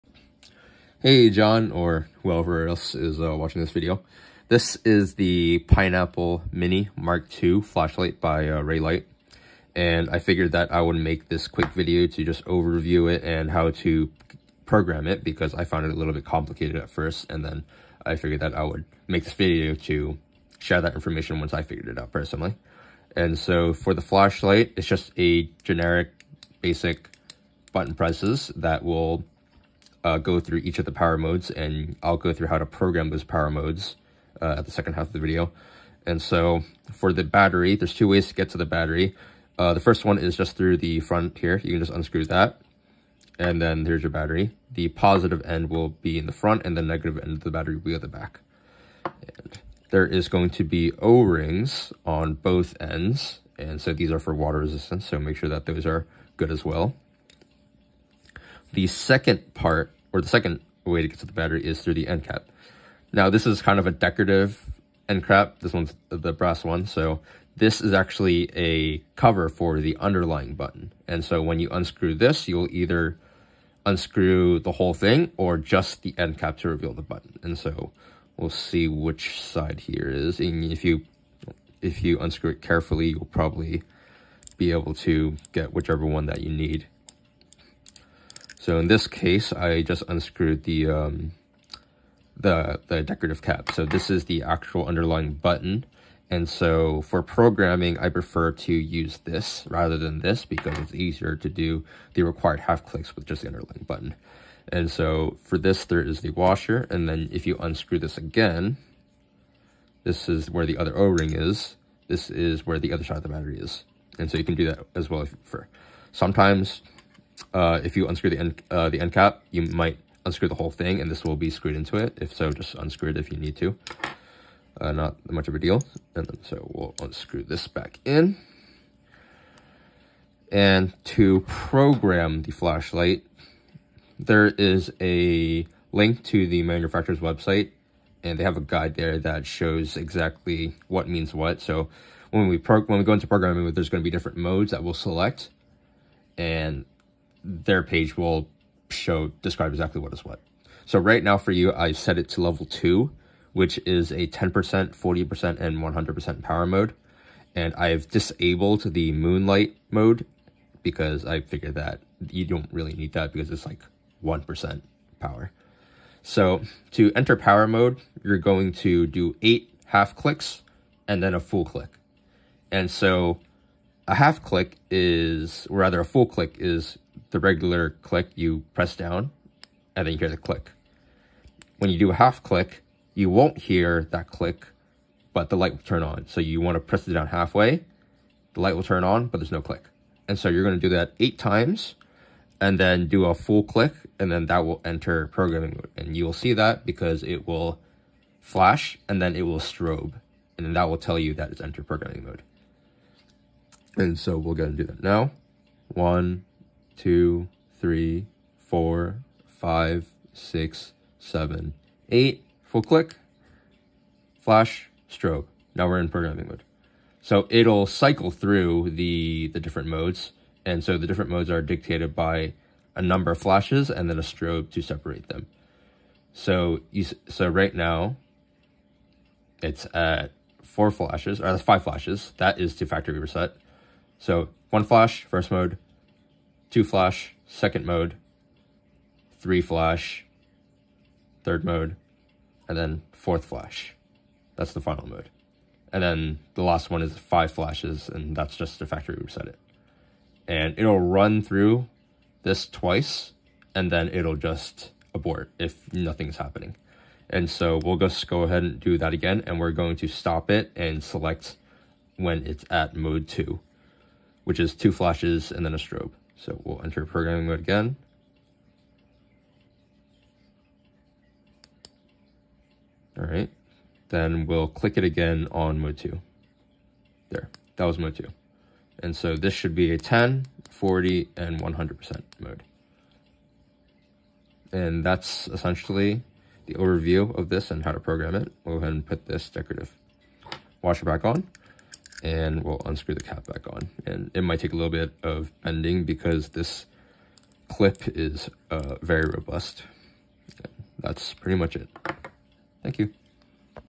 Reylight Pineapple Mini MKII - Overview/Disassembly & How to Program
Model Featured: Pineapple Mini MKII Brass-Frag